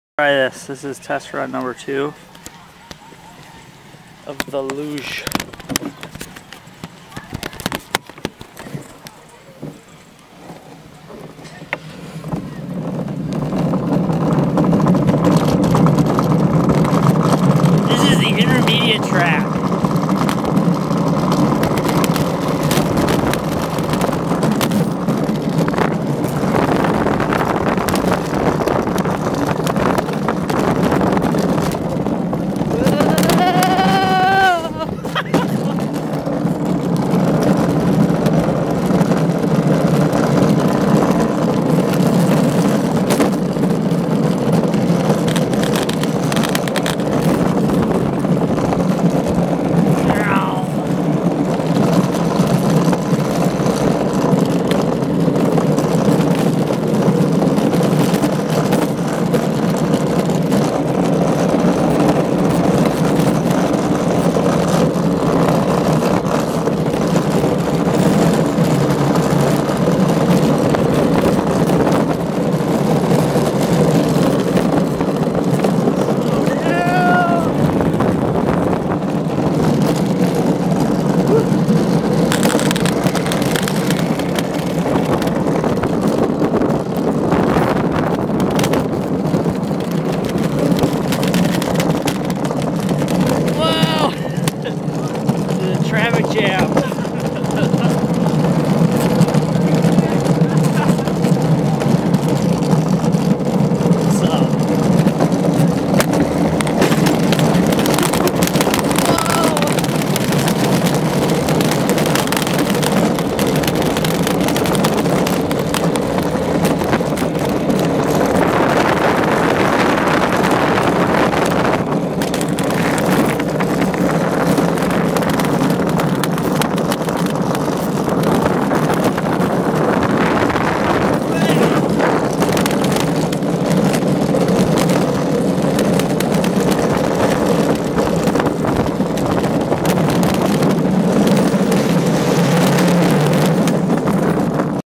luge!.AVI
Riding down a "luge" or alpine slide as we call them at home...

This is Alex riding down the intermediate track and is 1.5 KM long